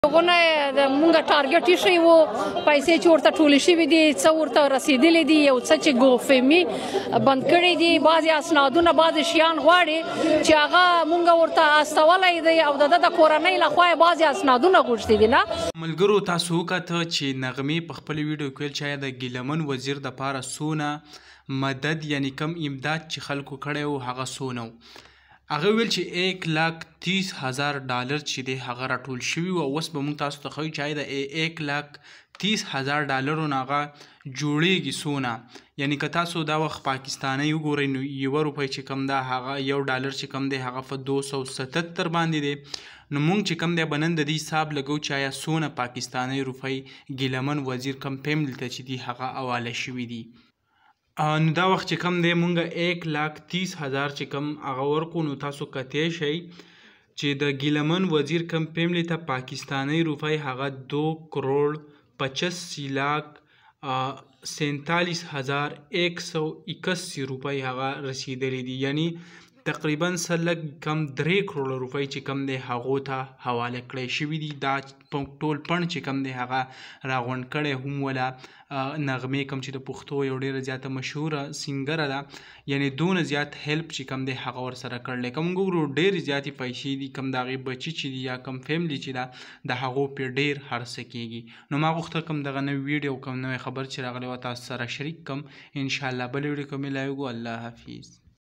Pashto singer Naghma Interview ma gilaman wazir sara $103000 help wako
pashto talk new video famous singer Naghma da gilaman wazir sara so dollar help wako
Naghma song